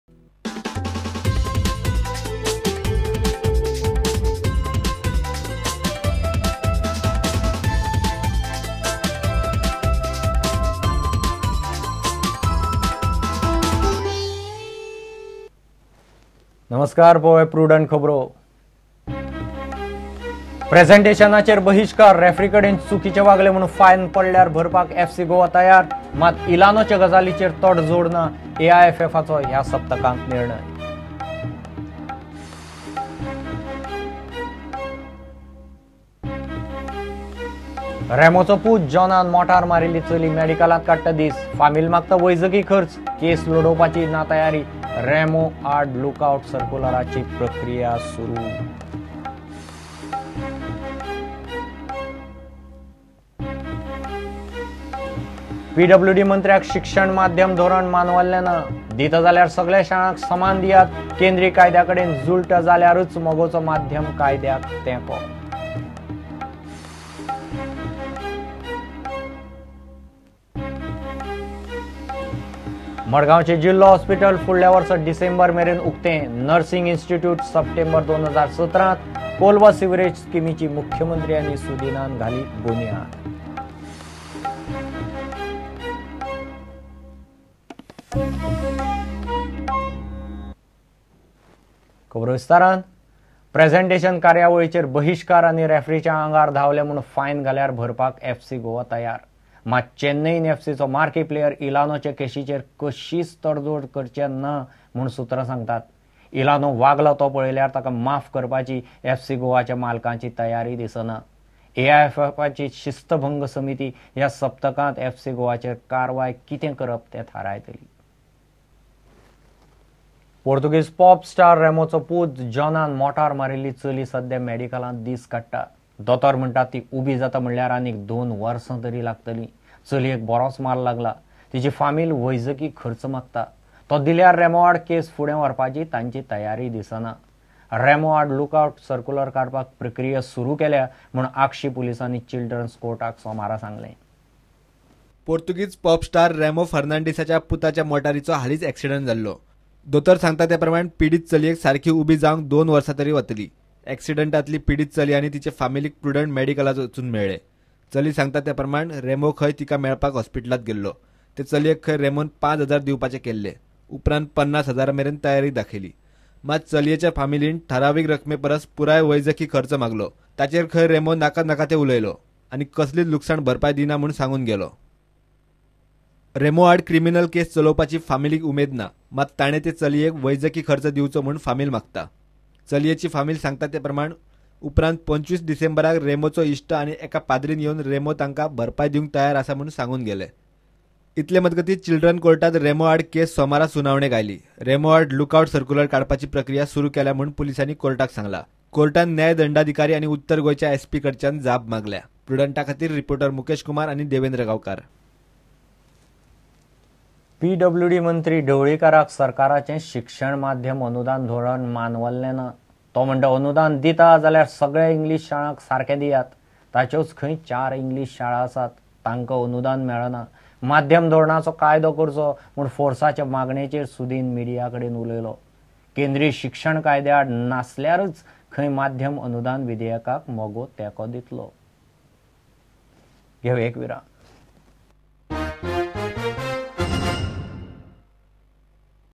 Prudent Media Konkani Prime News 281215 Part 1
FC Goa ready to pay penalty for boycotting ISL presentation ceremony, not ready to compromise on Elano's episode, AIFF decision this week